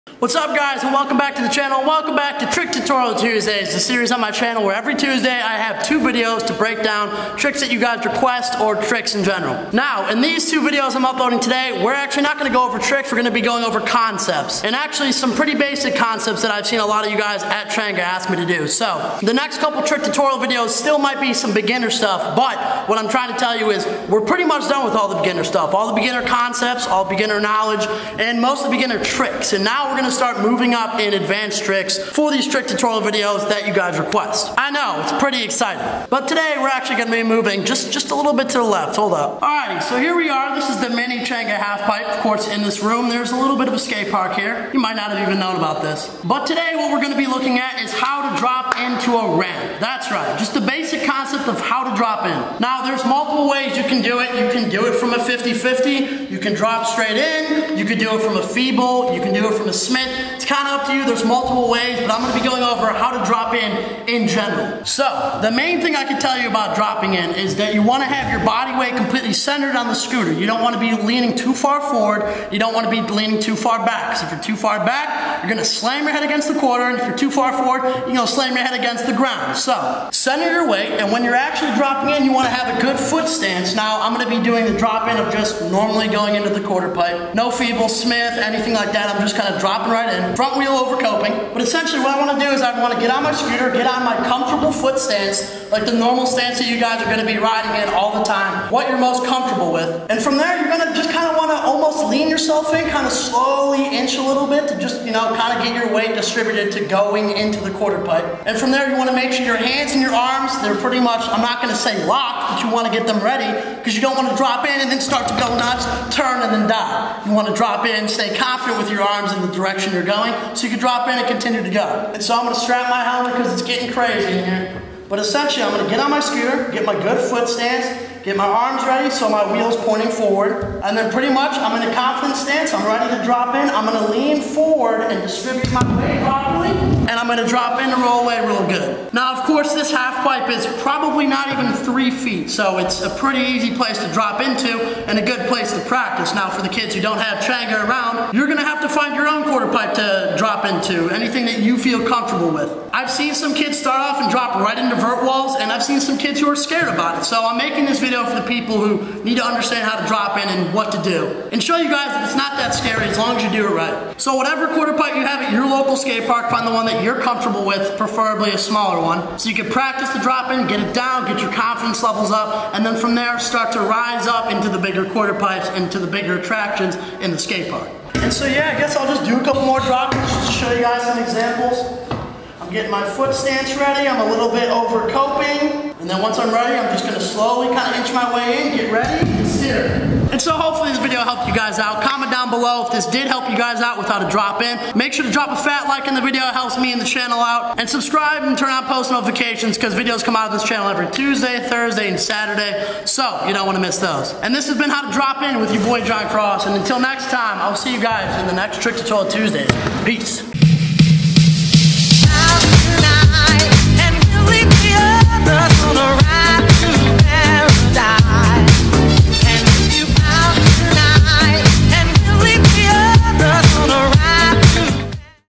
HOW TO DROP-IN ON A SCOOTER! Trick Tutorial Tuesdays
Whatsup guys and welcome back to one of the last basic trick or basic concept tutorial left! Soon I am going to start focusing on harder tricks!

100 LIKES FOR THIS TUTORIAL?!